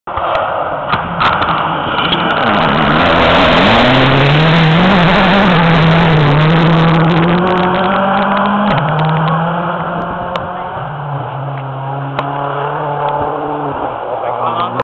WALES RALLY GB 2009. RHONDDA SUPER STAGE 10
HENNING SOLBERG ALMOST LOSING HIS CAR